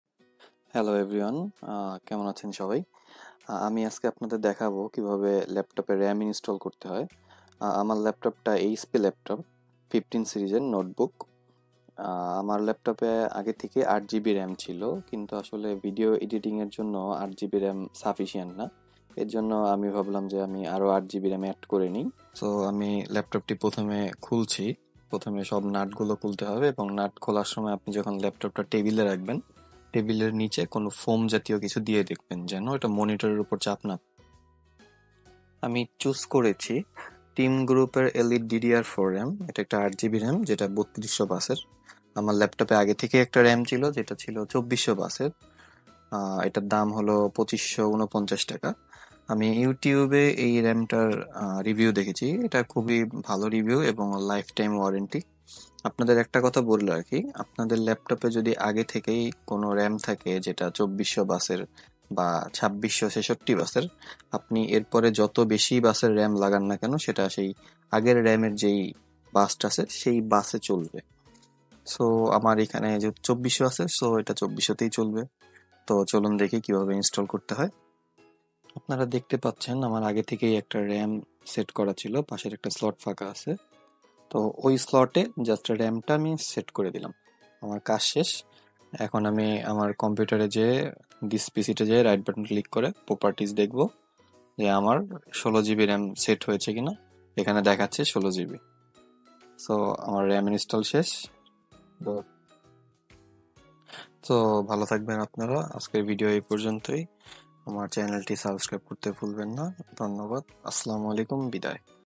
How to install Ram in your Laptop in Bangla | full installation guide I HP, ASUS LAPTOP
How to install Ram in your Laptop.
Looking to upgrade the RAM in your PC? This step-by-step guide in Bangla will show you how to easily install RAM in your laptop. Watch this video to learn the proper installation process and improve your computer's performance.